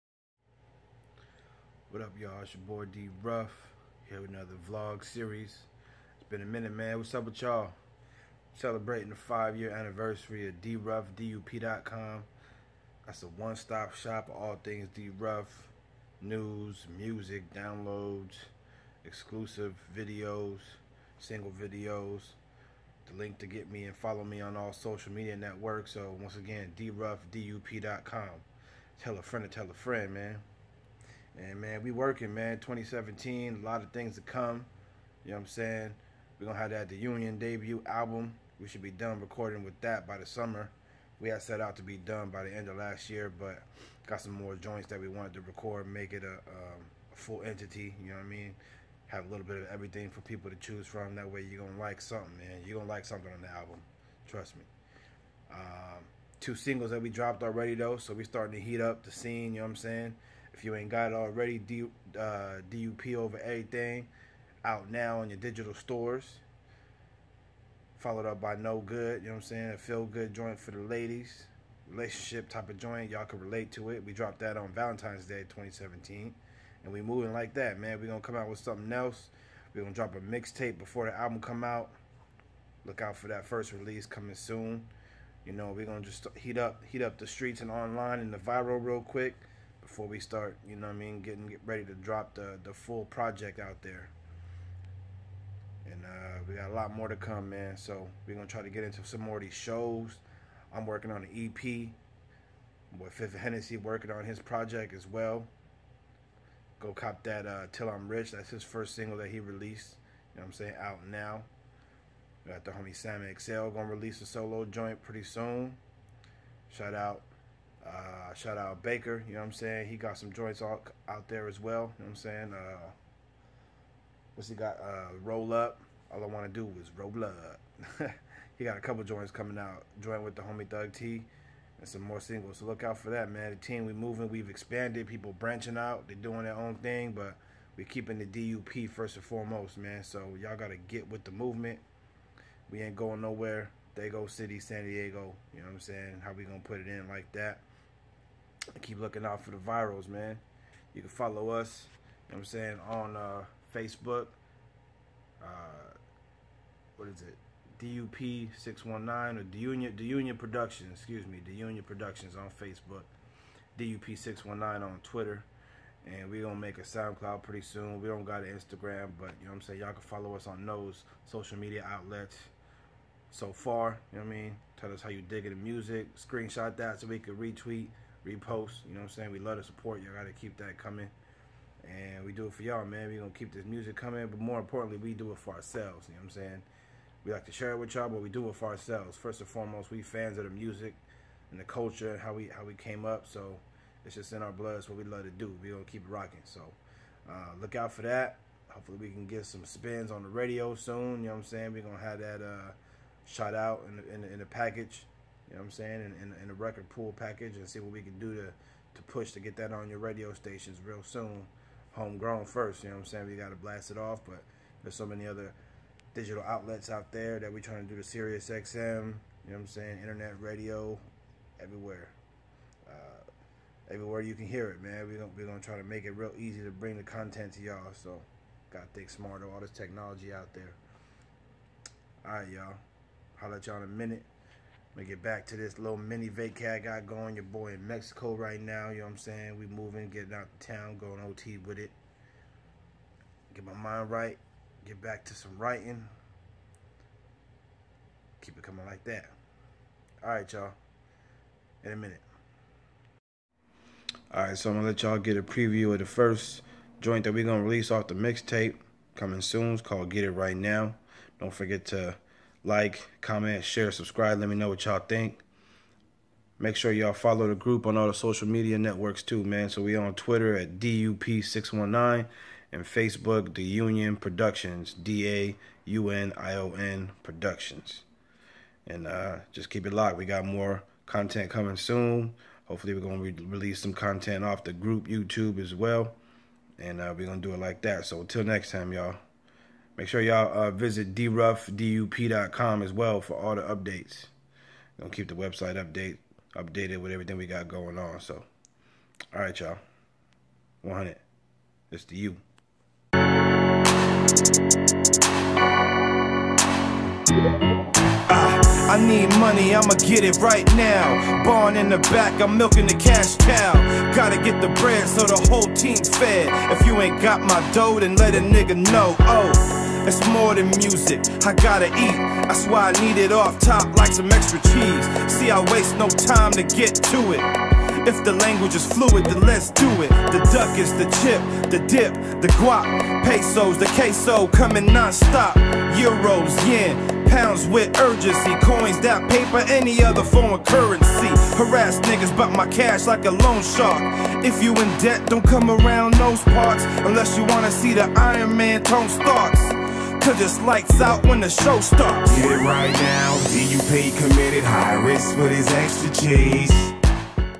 Dup project update ep.1
Whatup ya'll here's an update on what we got in the works with the team. I know i been m.i.a. on here. That's going to change. Follow the group on twitter @dup619 and fb da union productions.